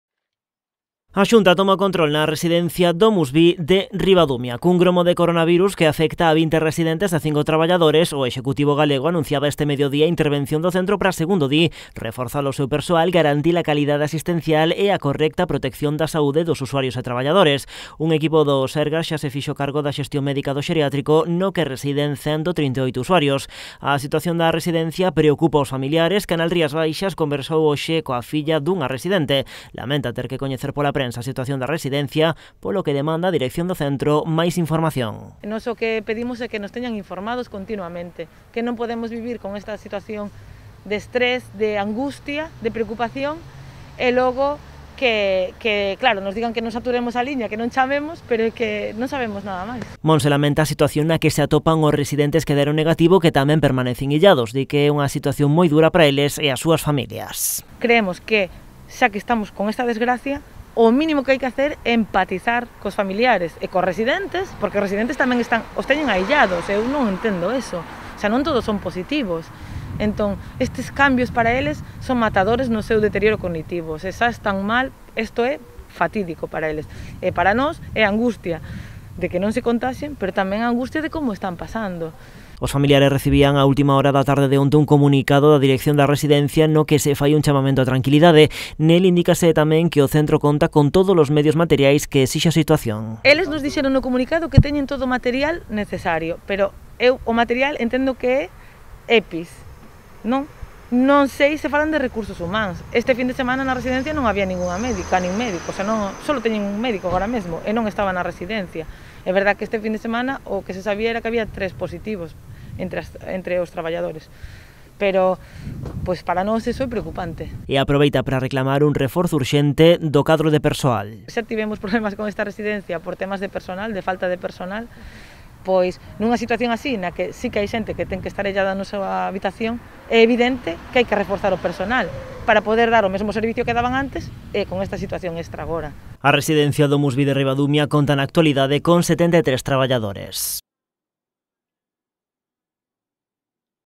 A Xunta toma o control da residencia DomusVi de Ribadumia
A Xunta intervén a residencia de DomusVi de Ribadumia ante o gromo de coronavirus detectado no centro. Un equipo do SERGAS asumiu este mediodía a súa xestión médica co obxectivo, di o goberno galego, de garantir a calidade asistencial e a correcta protección da saúde, tanto dos residentes como dos traballadores. Os familiares demandan máis información por parte da dirección do centro.